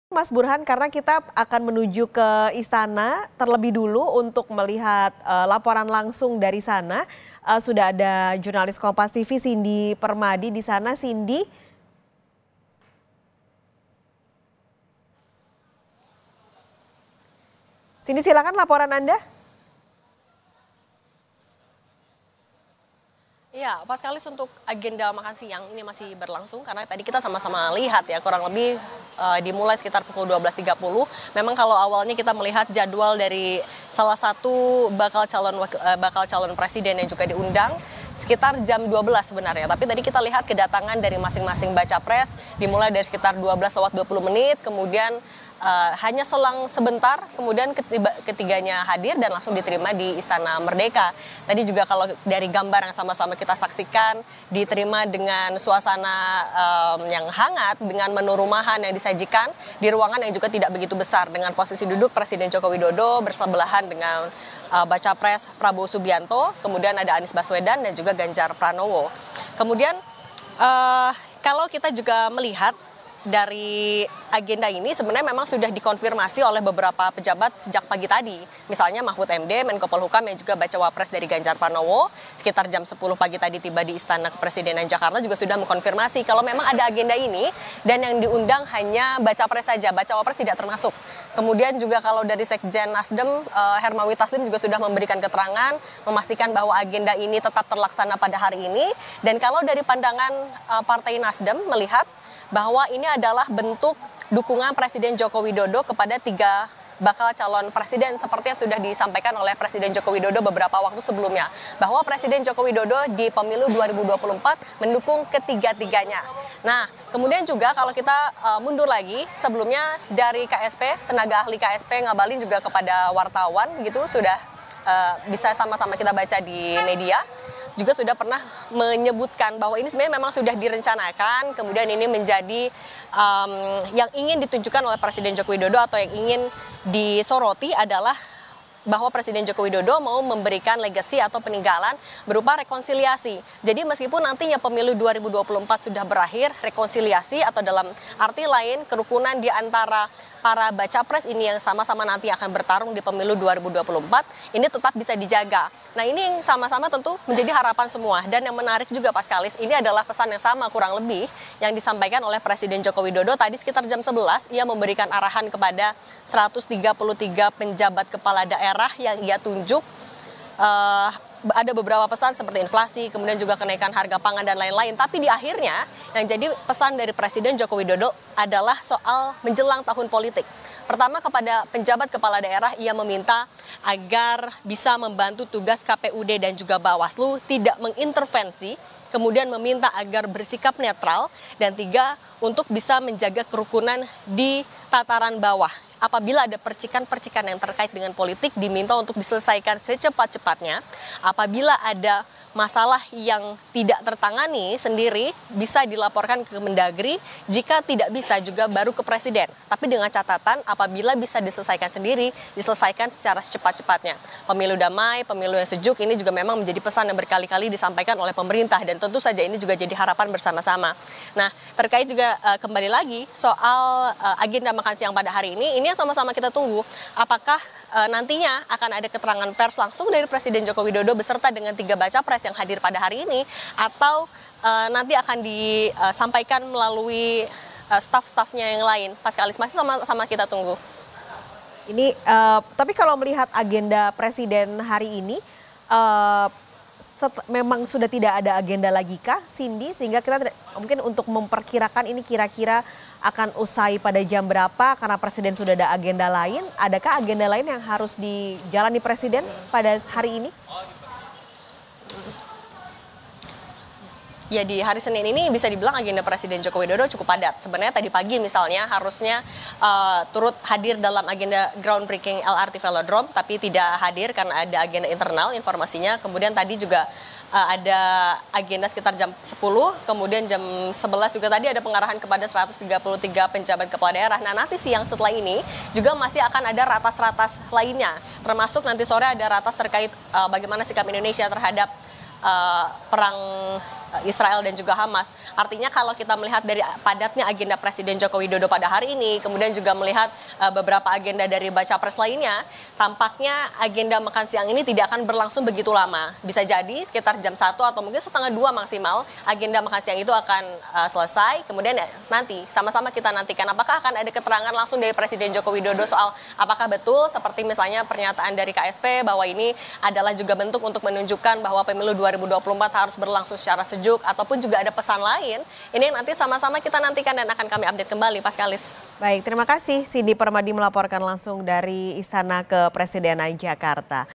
Suasana Makan Siang Bareng Jokowi dengan 3 Bacapres: Anies, Ganjar dan Prabowo
JAKARTA, KOMPAS.TV - Presiden Joko Widodo hari ini (30/10) bertemu dan menggelar makan siang bersama dengan tiga bakal calon presiden.

Ketiga bakal capres itu diundang Jokowi, tanpa bakal cawapres pendamping mereka.

Pada Senin pagi, Presiden Joko Widodo, memberikan pengarahan kepada ratusan penjabat Kepala Daerah di Istana Kepresiden Jakarta.

Usai pertemuan dengan para penjabat kepala daerah, Jokowi akan bertemu dan menggelar makan siang bersama tiga bakal calon presiden.